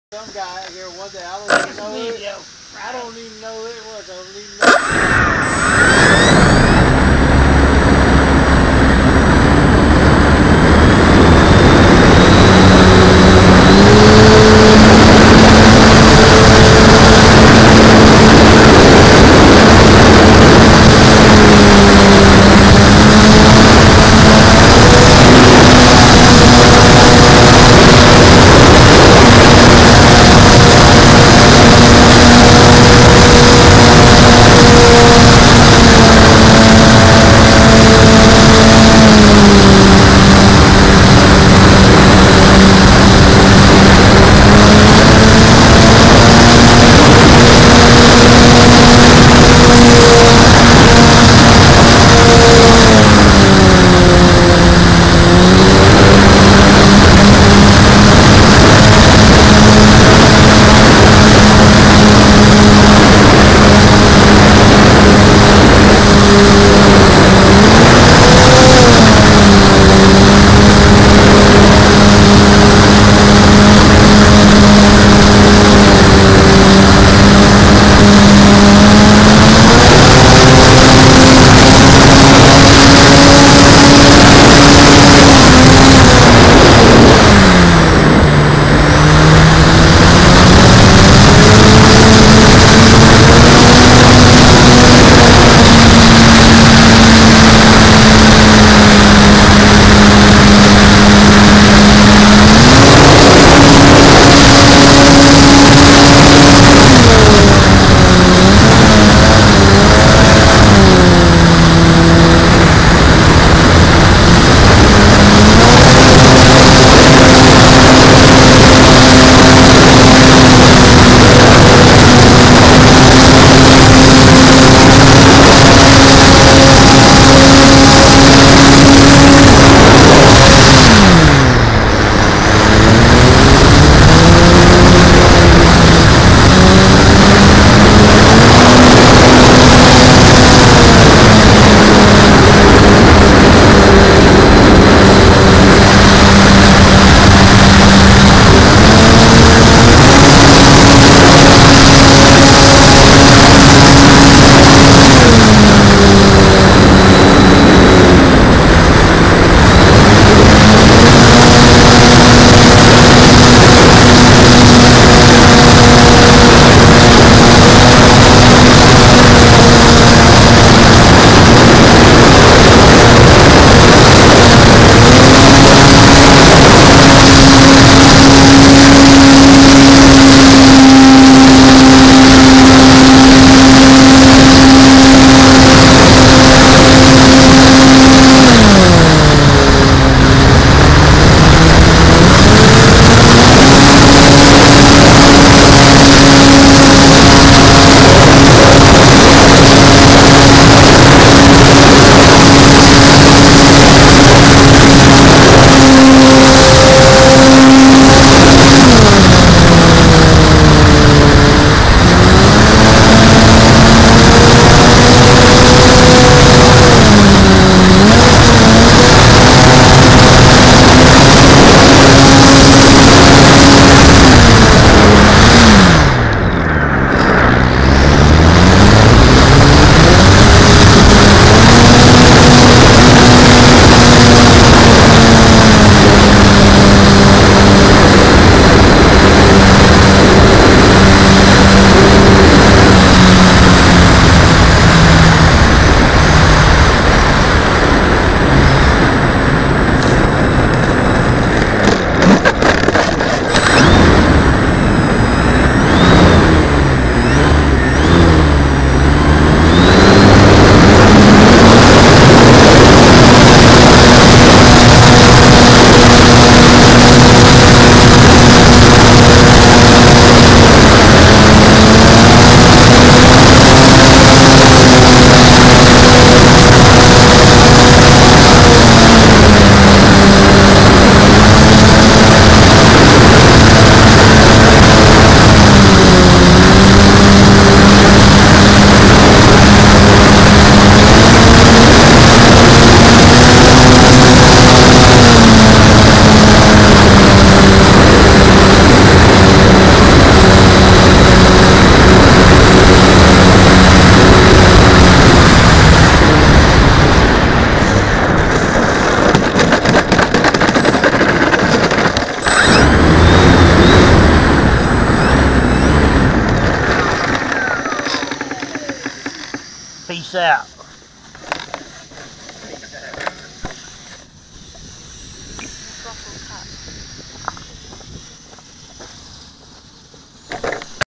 Saturday morning at the Dalton RC field
Ryan putting the Tundra thru it's paces above the freshly mowed field!